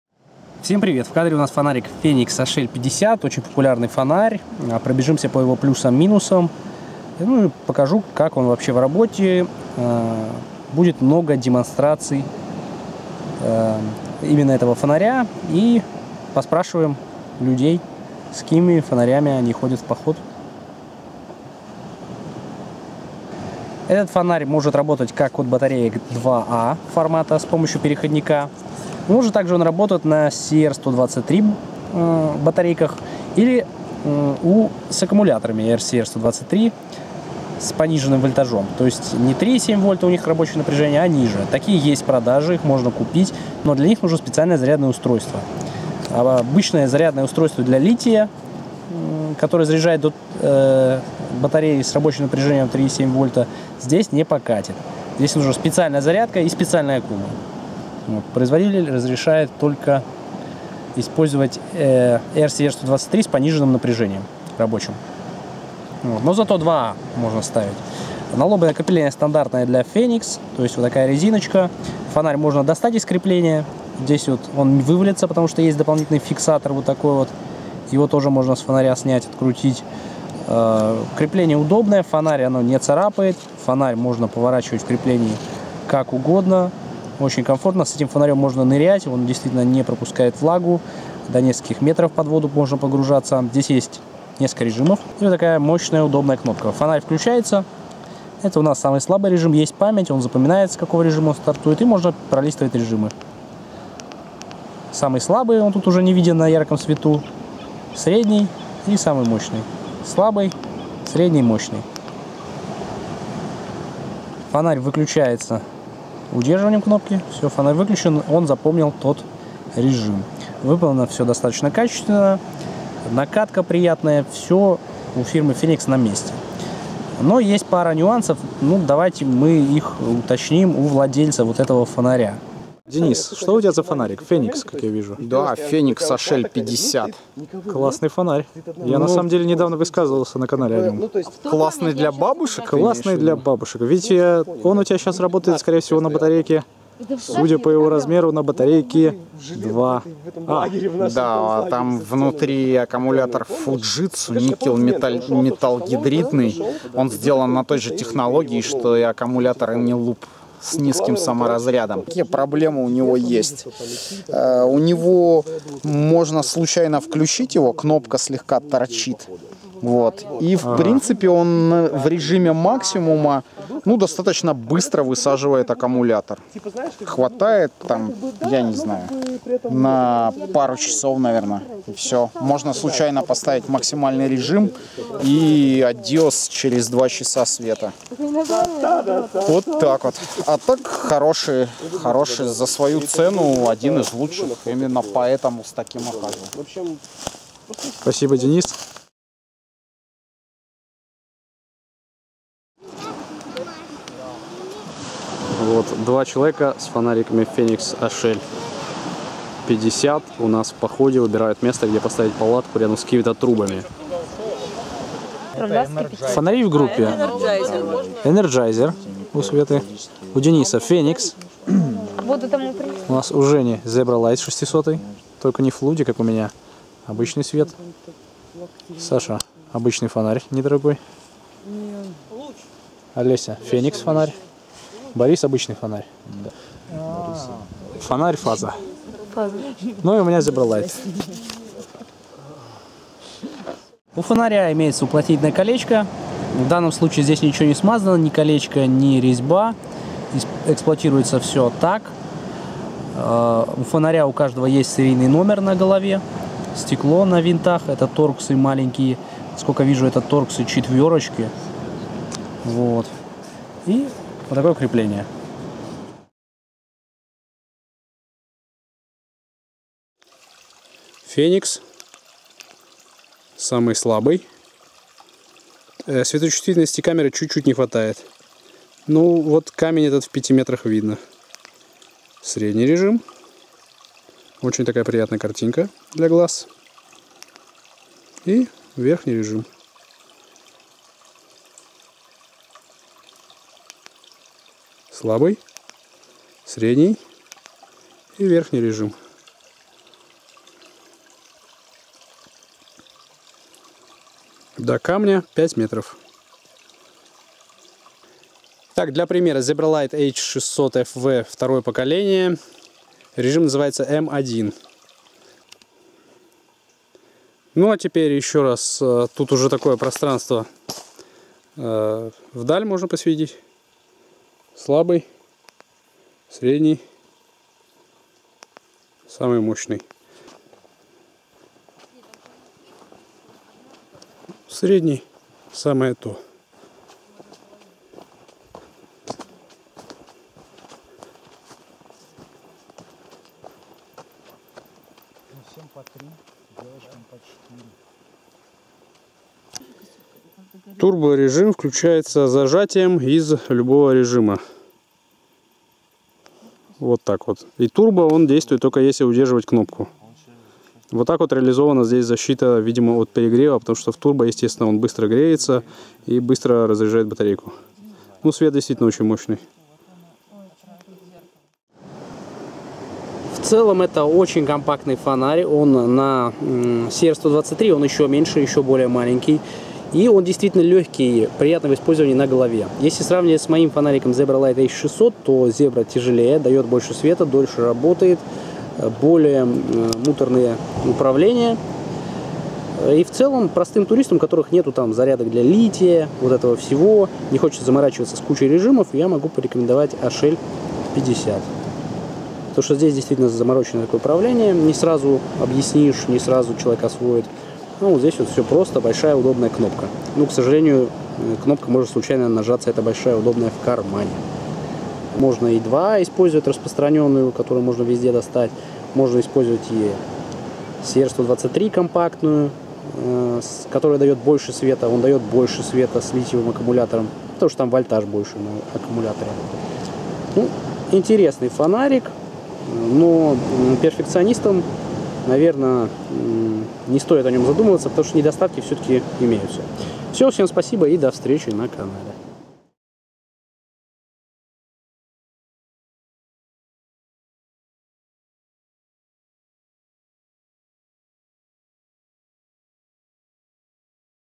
Налобный фонарик для похода Fenix HL50
Налобник Fenix HL50, популярная, распиаренная модель от Феникс. Если не смущает слабый источник питания и отсутствие блокировки кнопки, имеются плюсы - компактность, простое управление. Денчик, счастливый владелец
Размеры корпуса фонаря - 63,8*32мм. Вес с аккумулятором АА: 63 гр. Крепление весит ещё 32гр.
На АА выдает: 3-55-150 lm + 285lm turbo. (2 часа -6 часов -110 часов).
На CR123 и RCR123 (3.0V): 4-60-170 lm + 365 lm turbo. (3 часа - 10 часов -150 часов).
Светодиод Cree XM-L2 (Т6).